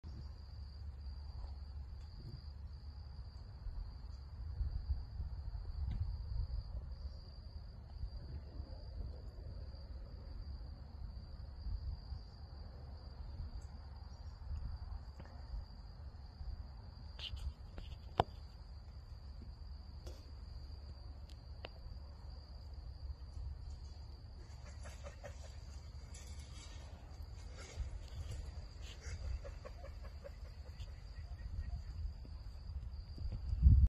Ezra vs Vulture
Shhhh! Be very, very quiet. He’s huntin’ birds. Poor Ezra. He may be sneaky, but he’s not faster than the turkey vultures trying to get a free snack from his dinner.